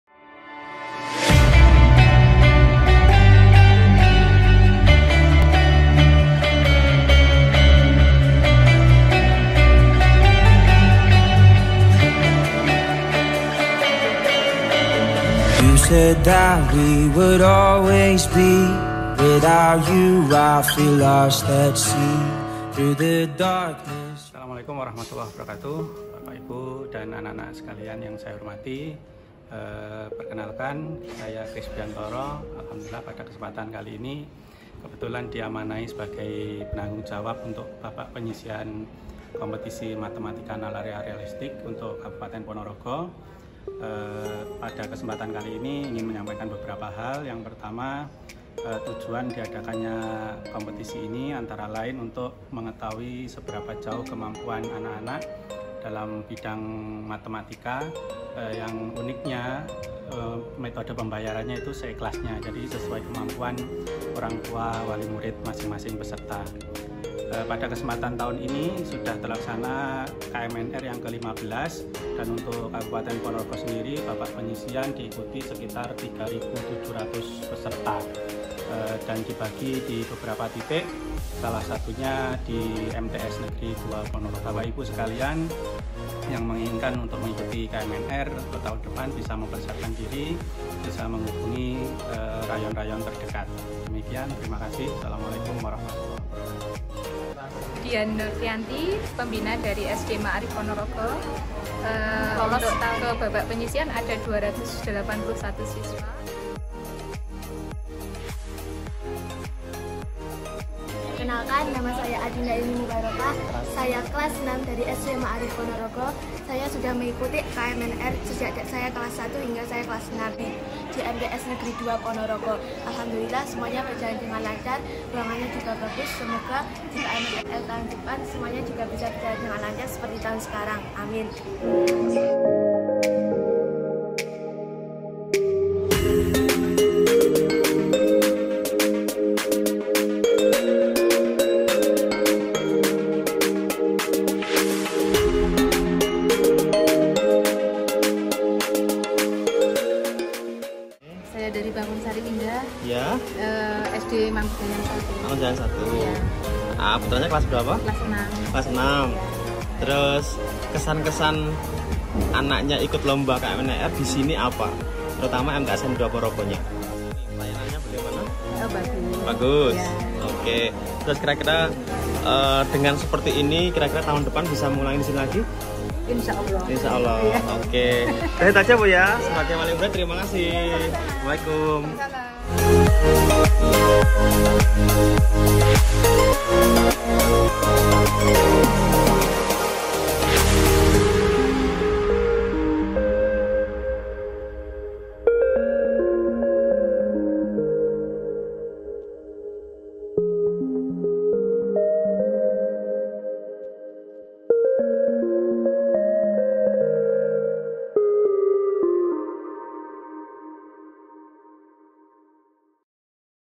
MTsN 2 Ponorogo, sebagai tuan rumah Seleksi babak penyisihan KMNR Ke 15 Se Indonesia
Selamat datang dan berkompetisi peserta babak penyisihan KMNR ke-15 Se Indonesia di MTsN 2 Ponorogo, Minggu 24 Nopember 2019.